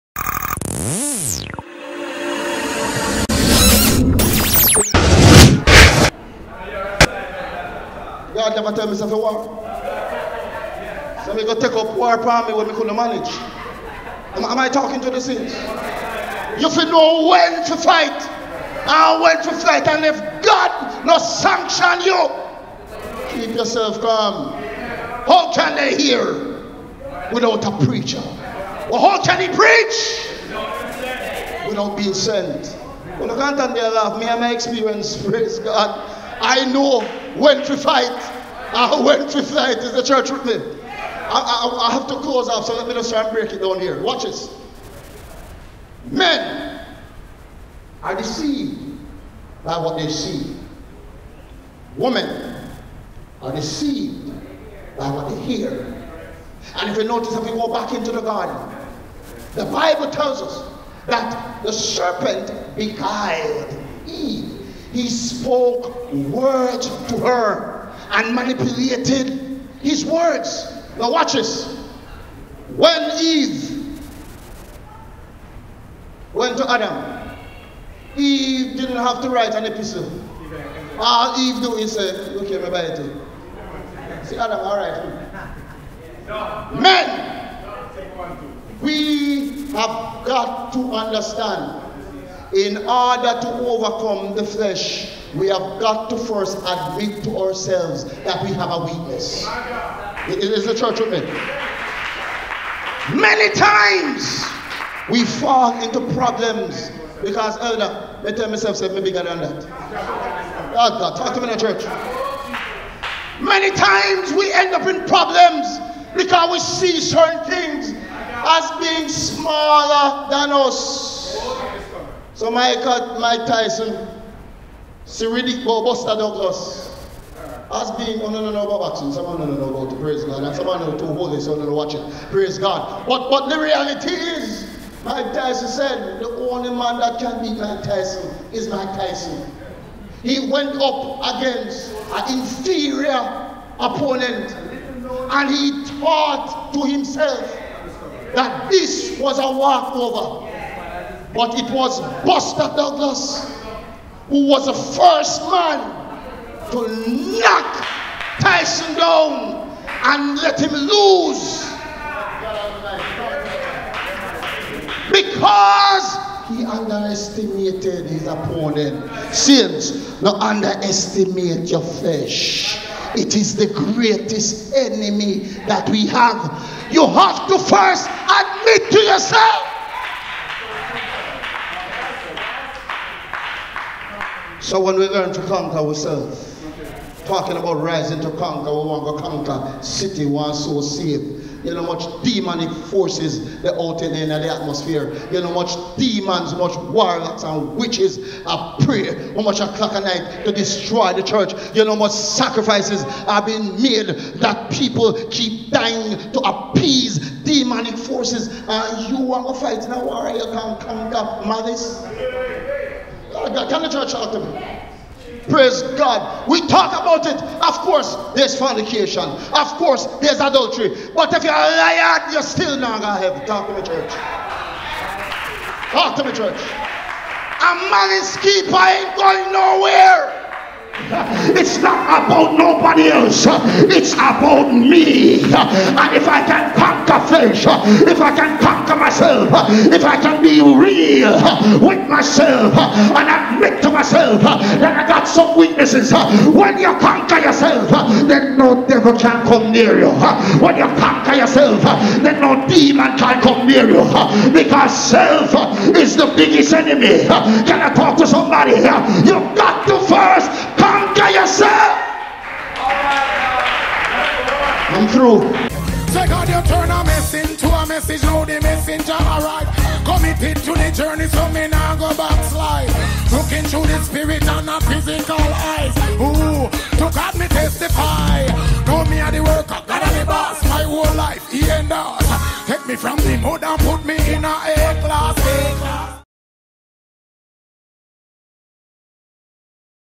HBAC CONVOCATION 2018 MEN'S DAY
Man in the flesh can't please God..I was bless to be a presenter on men's day HBAC CONVOCATION 2018